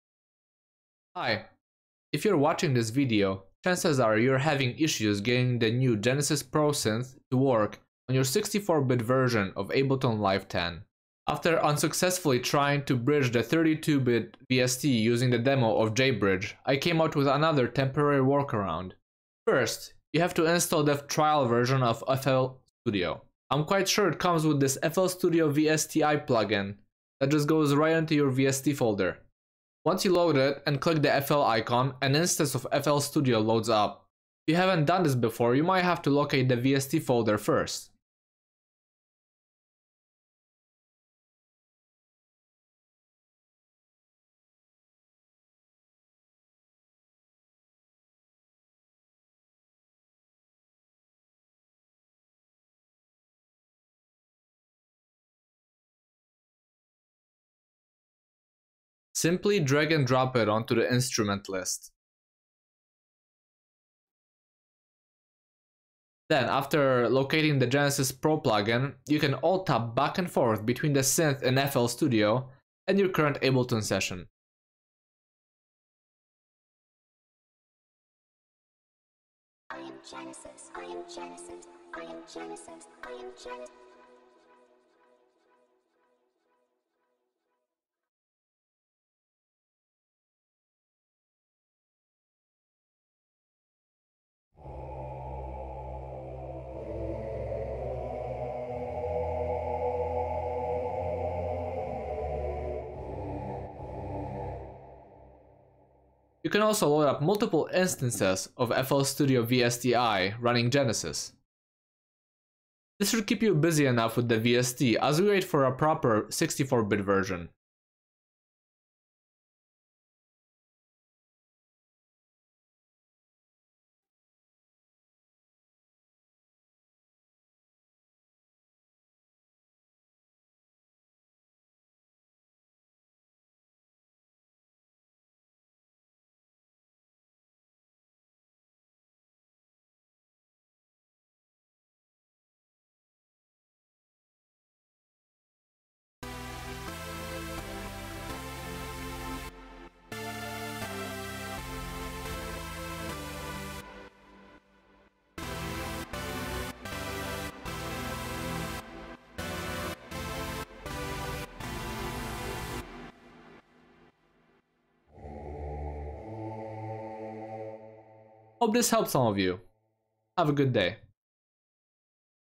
Guide: Genesis Pro on 64 bit Ableton Live 10 using FL Studio VSTi
Hi there, after fiddling with the Genesis Pro VST for a bit and not being able to get it to work on 64 bit version of Ableton Live 10 I figured out a small workaround that will let you use the synth thanks to FL Studio's VST bridging abilities. Hope this helped some of you!